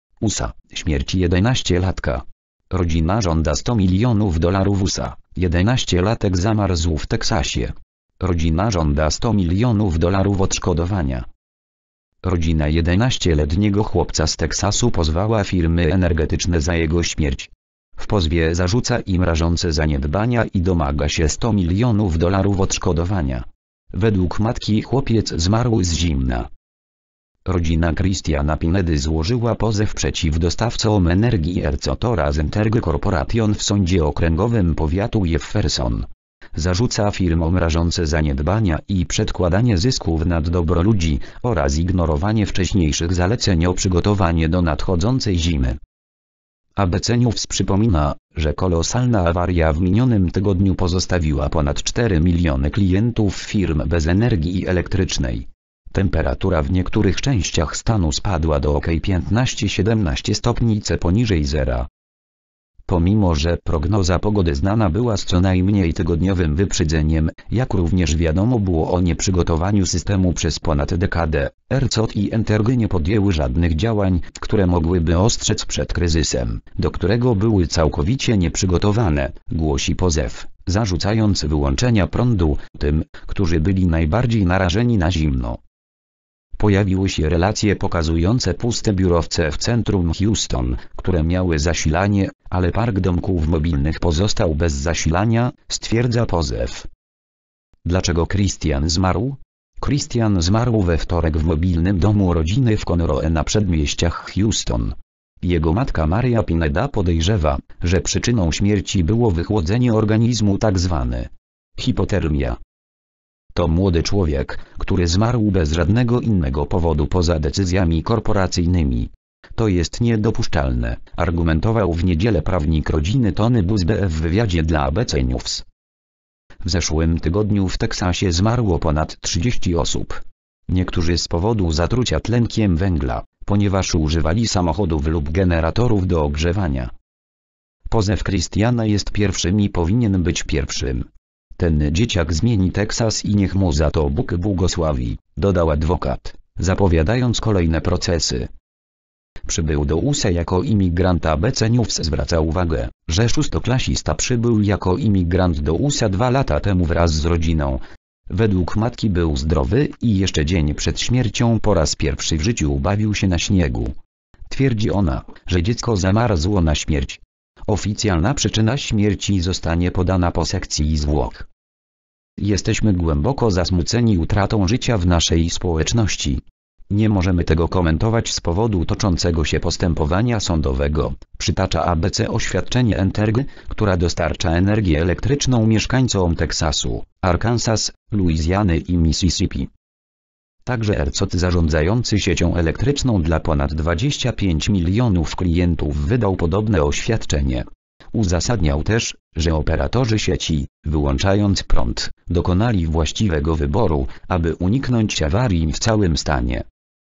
USA: Śmierć 11-latka. Rodzina żąda 100 mln dolarów.
USA: 11-latek zamarzł w Teksasie. Rodzina żąda 100 mln dolarów odszkodowania.

  (fot. private archive)